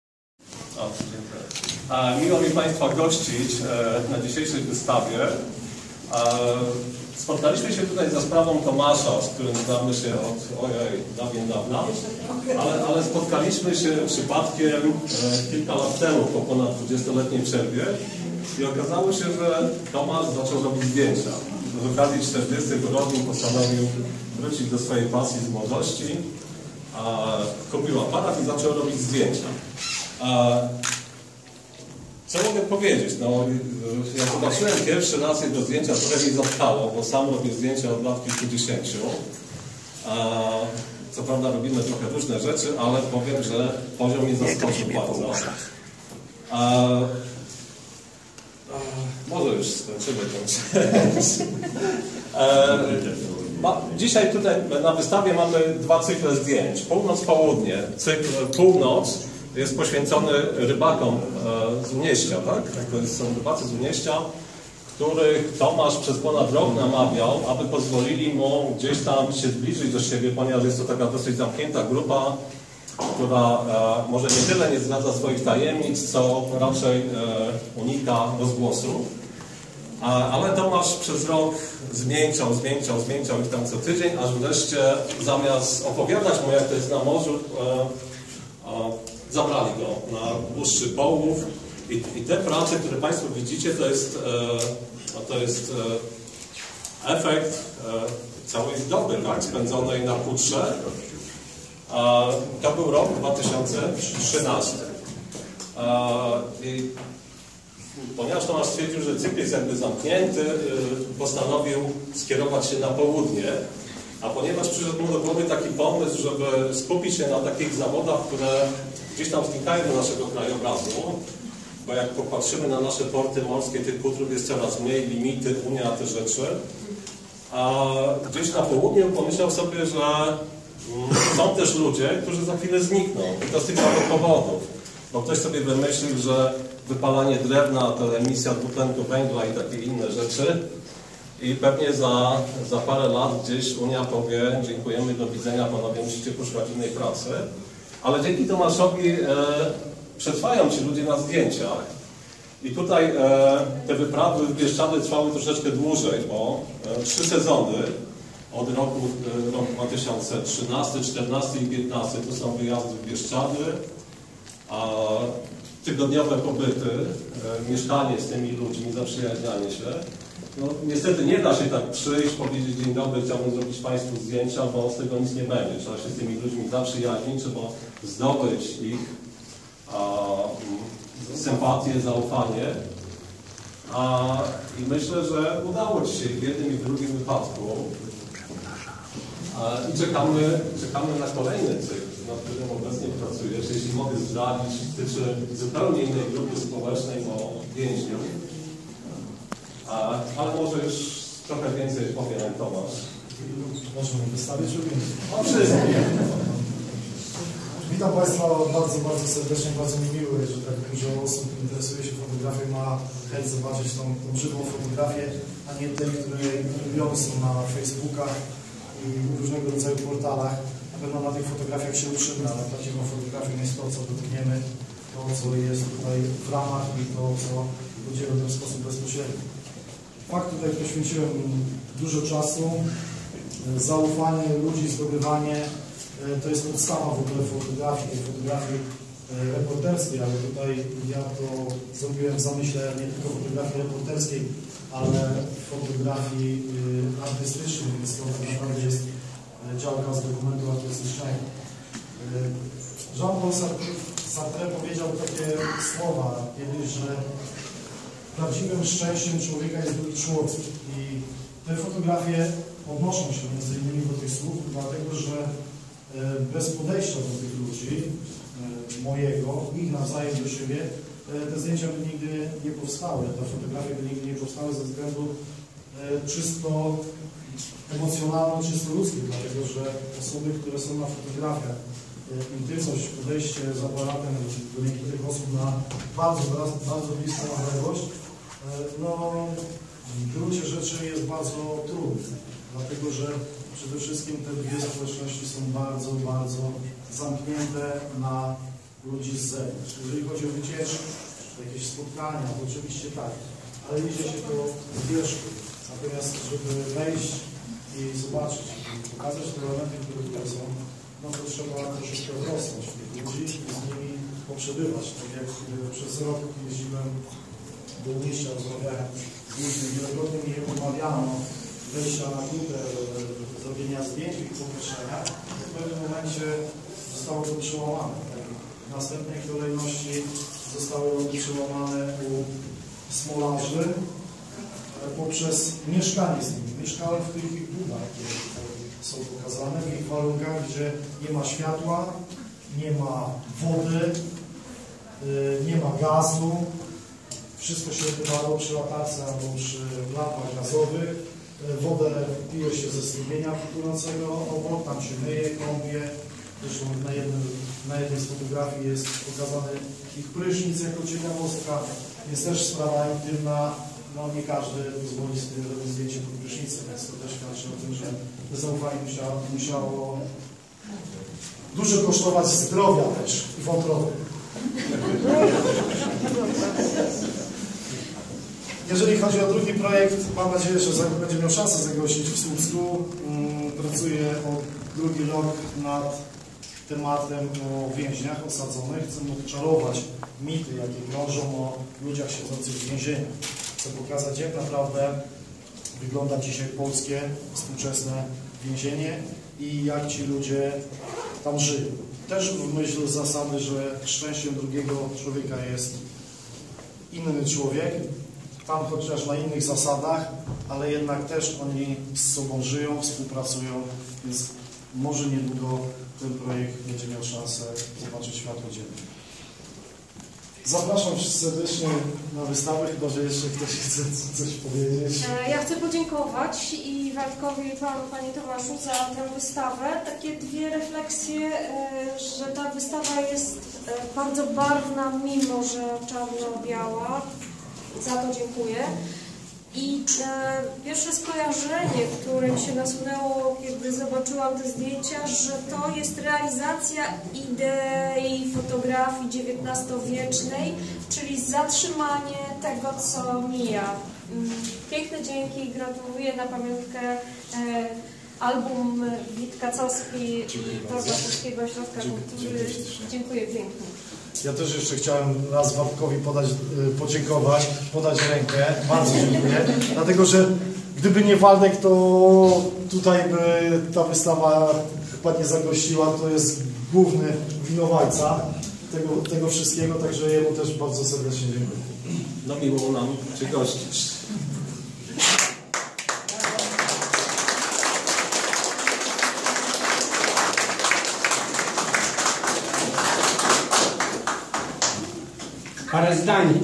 2016.04.18 Wystawa Tomasza Juszkiewicza
OT.Rondo w Słupsku otwarcie wystawy fotografii autorstwa Tomasza Juszkiewicza pt. PÓŁNOC POŁUDNIE -rybacy i smolarze